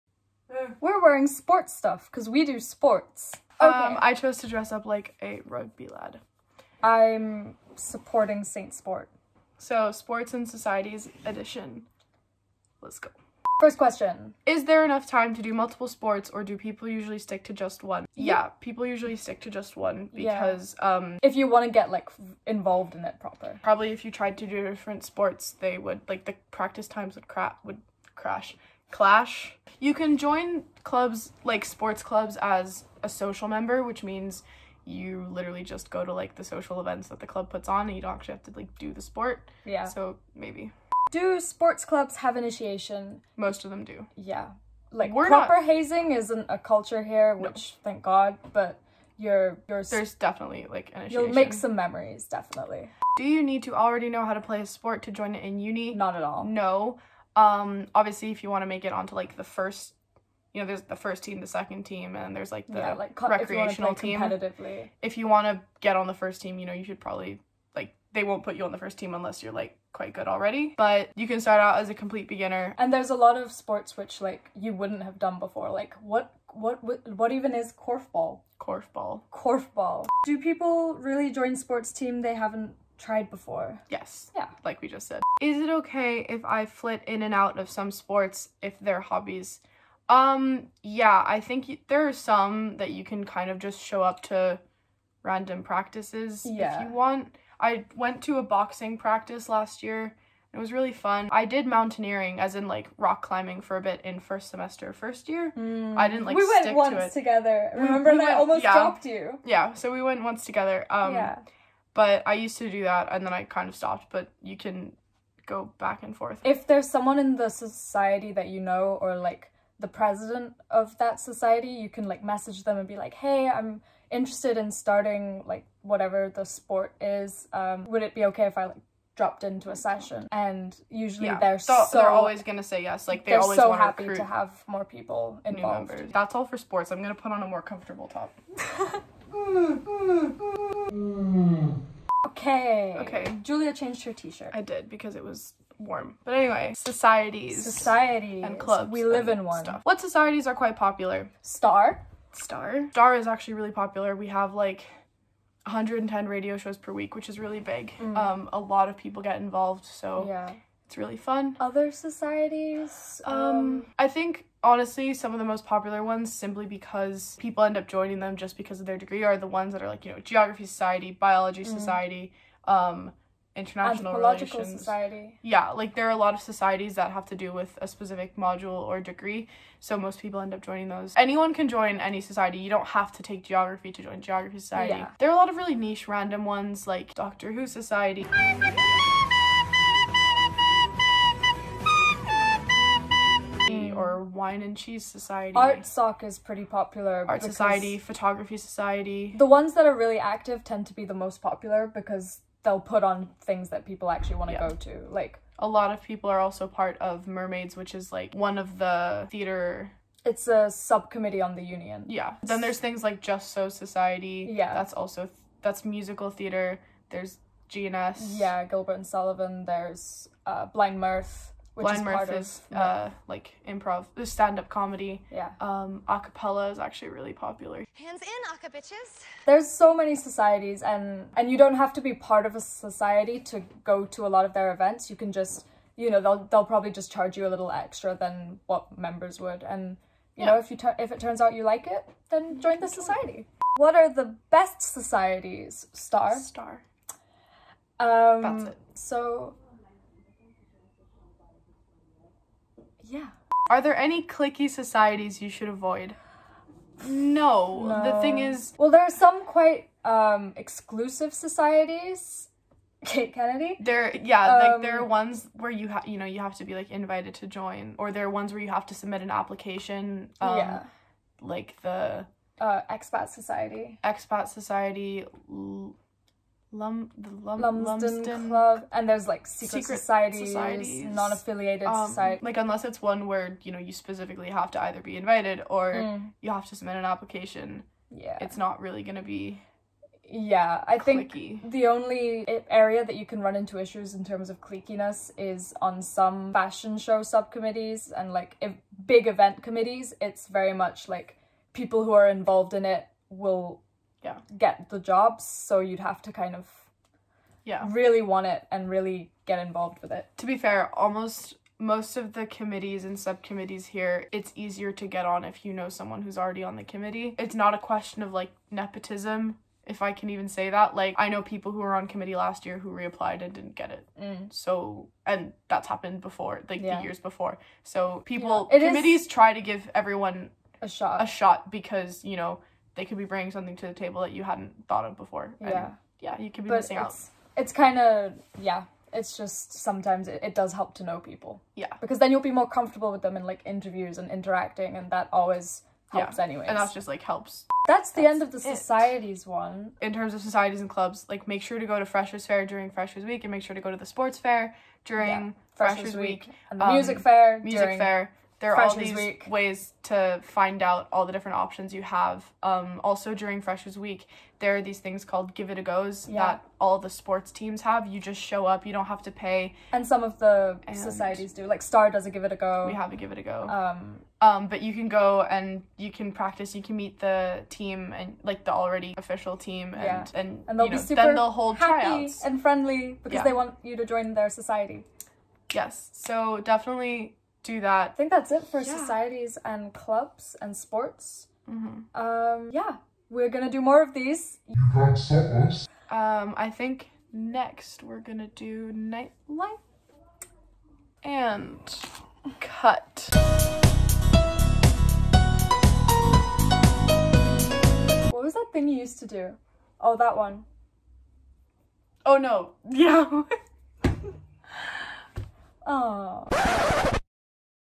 STAR VLOGS 5 - SPORTS AND SOCIETIES IN ST ANDREWS
Sports and societies edition let's go

Presented by Julia Swerdlow and Surina Martin
Edited by Surina Martin

Watch Responsibly.